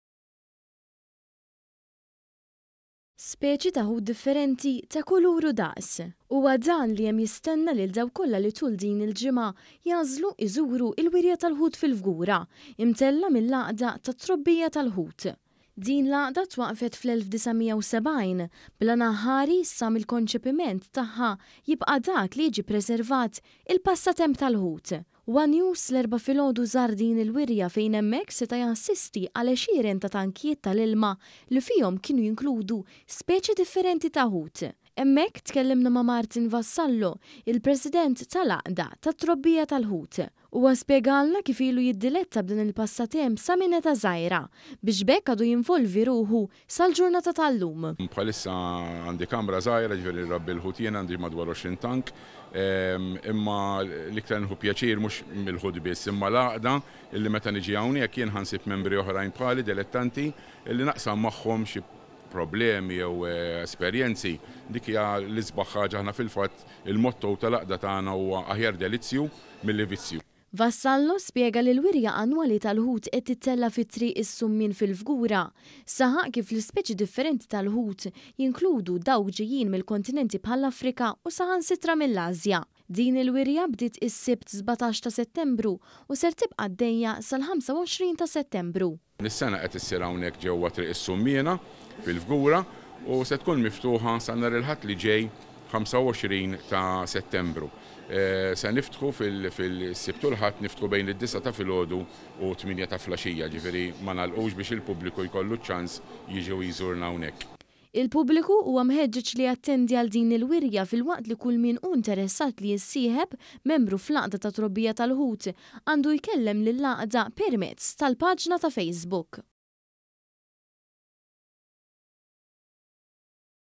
TISPIKKA WIRJA TAL-ĦUT GĦALL-PUBBLIKU FIL-FGURA
ONE.COM.MT • Bħalissa fil-Fgura qed tittella’ wirja tal-ħut mill-Għaqda tat-Trobbija tal-Ħut.
Fil-fatt, illum crew ta’ ONE News żar din il-wirja fejn hemmhekk seta’ jassisti għal numru kbir ta’ tankijiet tal-ilma li fihom kien hemm għexieren ta’ speċi differenti ta’ ħut ta’ kull kulur u daqs.

KITBA: NIRAYL GRECH
FILMATI: BJORN BONELLO

Speċi ta’ ħut differenti, ta’ kull kulur u daqs.

Huwa dan li hemm jistenna lil dawk kollha li tul din il-ġimgħa jagħżlu jżuru l-Wirja tal-Ħut, fil-Fgura mtella’ mill-Għaqda tat-Trobbija tal-Ħut.

Din l-għaqda twaqqfet fl-1970 bl-għan aħħari sa mill-konċepiment tagħha jibqa’ dak li jiġi ppriservat il-passatemp tal-ħut.

One News l-Erbgħa filgħodu żar din il-wirja fejn hemmhekk seta’ jassisti għal għexieren ta’ tankijiet tal-ilma li fihom kienu jinkludu speċi differenti ta’ ħut.

Hemmhekk tkellimna ma’ Martin Vassallo, il-President tal-Għaqda tat-Trobbija tal-ħut. Huwa spjegalna kif ilu jiddelitta b’dan il-passatemp sa minn età żgħira biex b’hekk għadu jinvolvi ruħu sal-ġurnata tal-llum.

MARTIN VASSALLO
PRESIDENT TAL-GĦAQDA TAT-TROBBIJA TAL-ĦUT
‘Bħalissa għandi kamra żgħira, għandu madwar għoxrin tank. Imma l-iktar li nieħu pjaċir mhux mill-ħut biss, imma l-għaqda illi meta niġi hawnhekk jien ħa nsib membri oħrajn bħali dilettanti illi naqsam magħhom xi problemi jew esperjenzi. Dik hija l-isbaħ ħaġa fil-fatt il-motto tal-għaqda tagħna huwa aħjar delizzju milli vizzju.’

Vassallo spjega li l-wirja annwali tal-ħut qed tittella’ fi Triq is-Summien fil-Fgura. Saħaq kif l-ispeċi differenti tal-ħut jinkludu dawk ġejjin mill-kontinenti bħall-Afrika u saħansitra mill-Asja.

Din il-wirja bdiet is-Sibt 17 ta’ Settembru u ser tibqa’ għaddejja sal-25 ta’ Settembru.

MARTIN VASSALLO
‘Din is-sena se ssir hawnhekk fi Triq is-Summiena fil-Fgura u se tkun miftuħa sa nhar il-Ħadd li ġej 25 ta’ Settembru. Se niftħu s-Sibt u l-Ħadd, niftħu bejn id-disgħa ta’ filgħodu u t-tmienja ta’ filgħaxija, iġifieri ma’ nagħlqux biex il-pubbliku ikollu ċ-ċans li jiġi u jżurna hawnhekk.’

Il-pubbliku huwa mħeġġeġ li jattendi għal din il-wirja filwaqt li kull min hu interessat li jissieħeb membru fl-Għaqda tat-Trobbija tal-Ħut, għandu jkellem lil għaqda peremzz tal-Paġna ta’ Facebook.